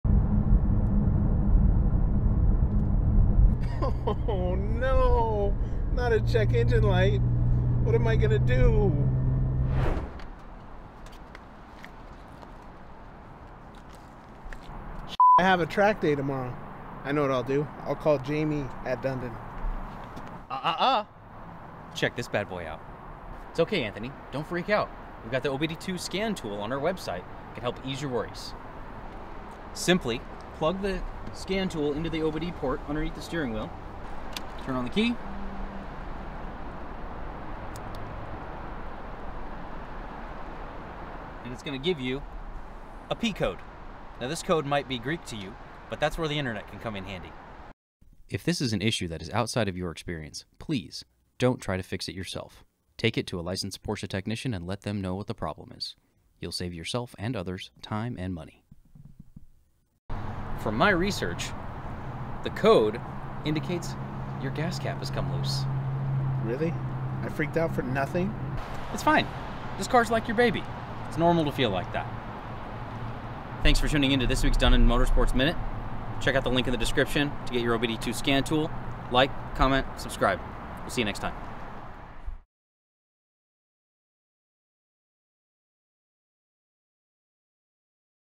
What to Do if Your Check Engine Light Turns on - Dundon Motorsports Minute
Ever have your check engine light come on at the worst time? Stay calm, with this video you'll be able to diagnose the problem yourself.

Special Thanks to Ted Anthony and Anthony Butay

Produced by Eric Rosu